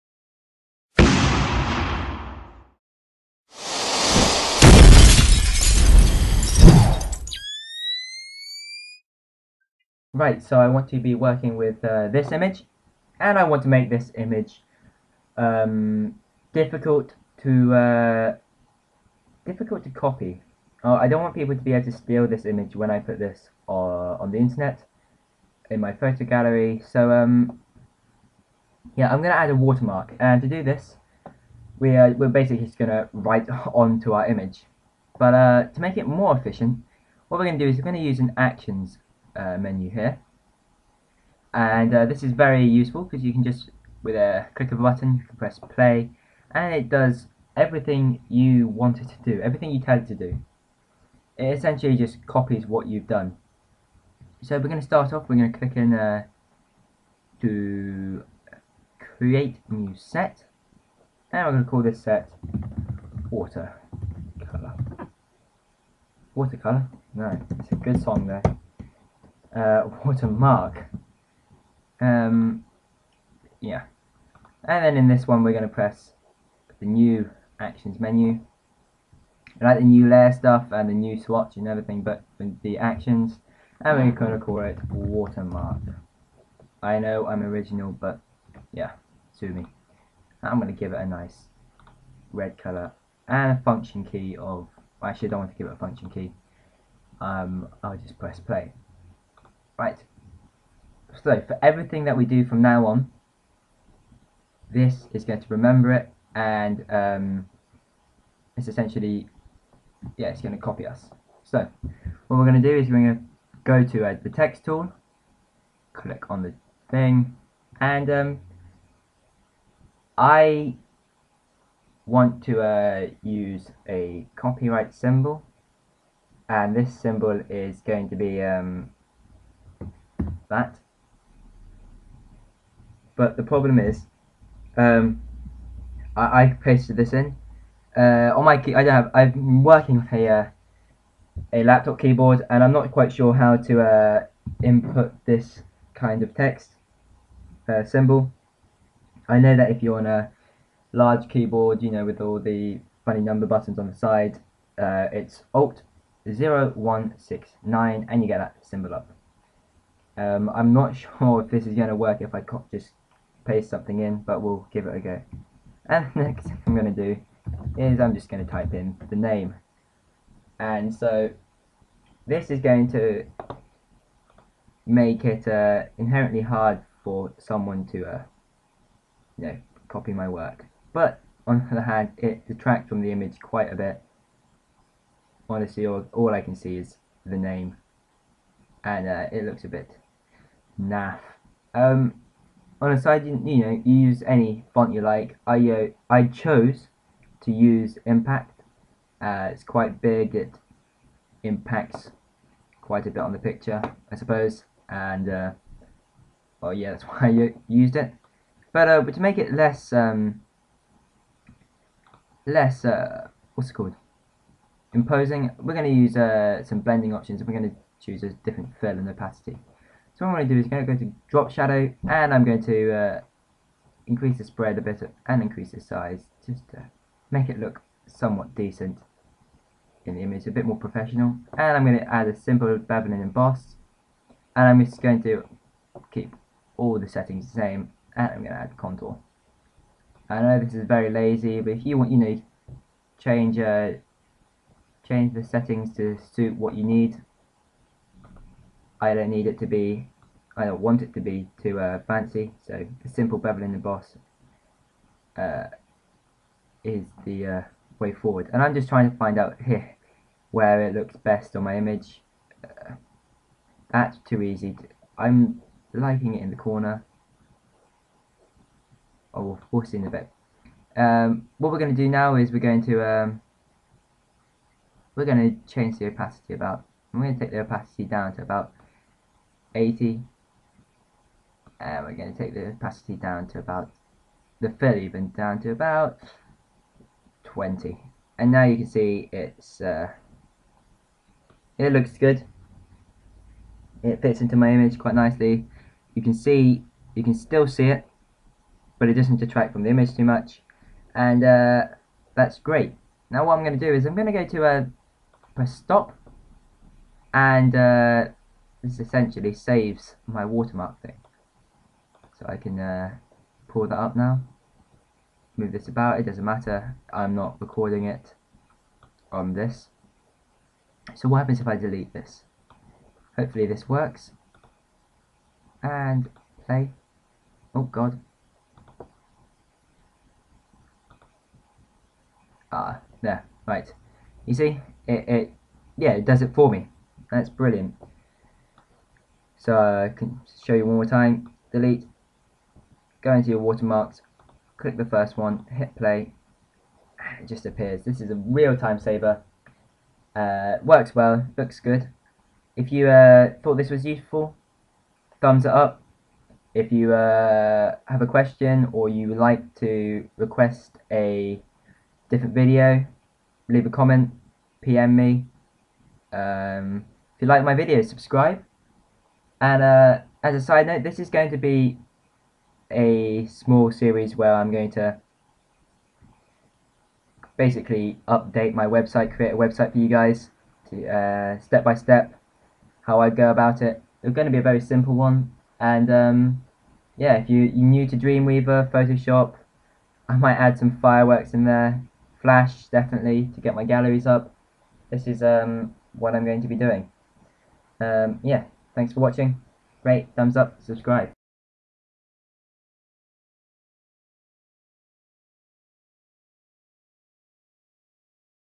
Protect Your images in photoshop
Easy way to protect your images on the internet